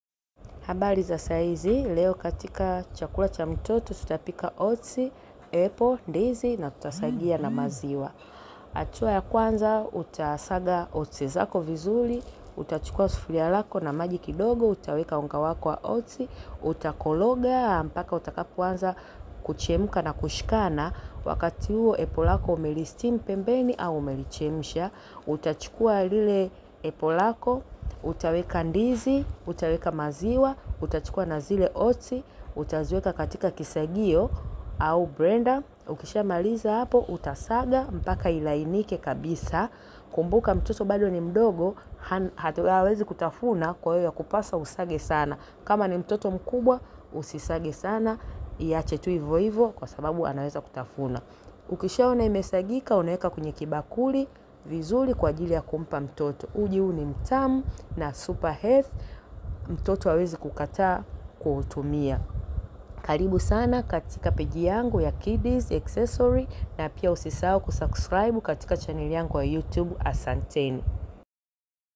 6monthsbabyfoodrecipes babyhealthy
mpe mtoto vyakula mbalimbali ili kuendelea kuimarisha afya yake na kumfanya asichukie chakula